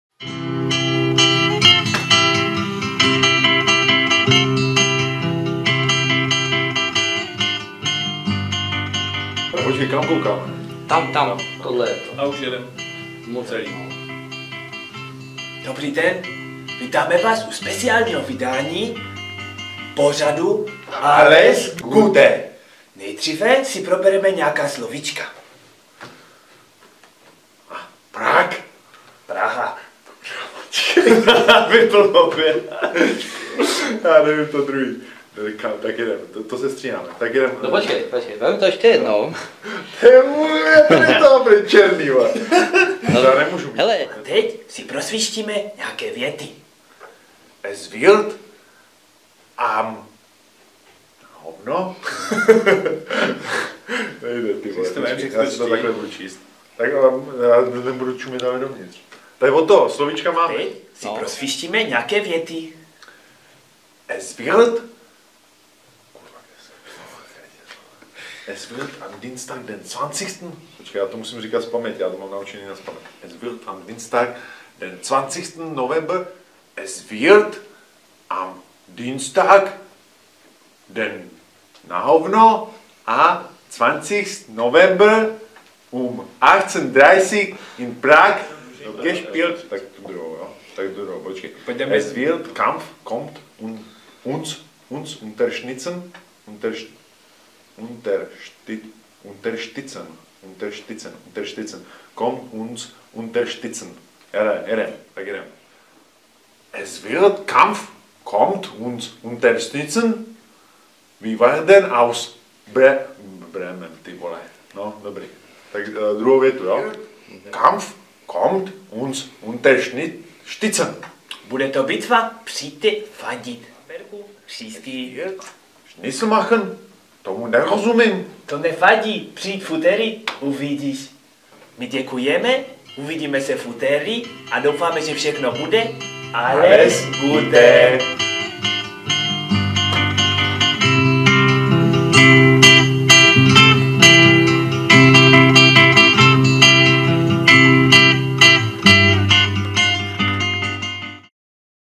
Bamberg 2
Petr po natáčení řekl: "To byl můj první kontakt s němčinou!!"

My jsme pro vás ještě připravili bonusové video, abyste věděli, že natáčení neproběhlo hned "na první dobrou".